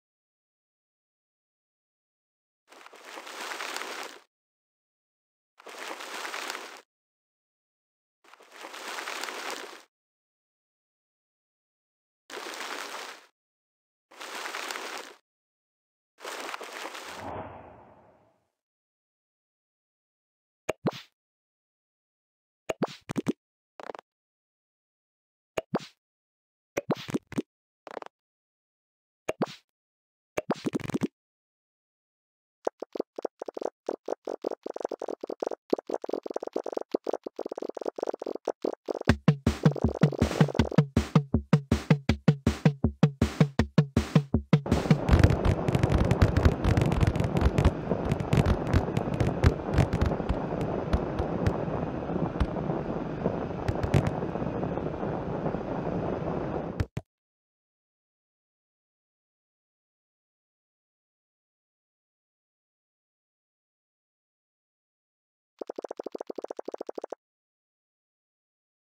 Juicy Run | Relaxing | ASMR 🍇🍈🍉🍊🍋🍌🍍🍎🍏🍐🍑🍒🍓🍅🌽asmr
This video is completely dedicated to our beloved corn! We will first expose it, and then we will see that not all residents are equally happy, we will quickly remove the sad locals, replacing them with cheerful ones, and after that we will roast all parts of our corn well ...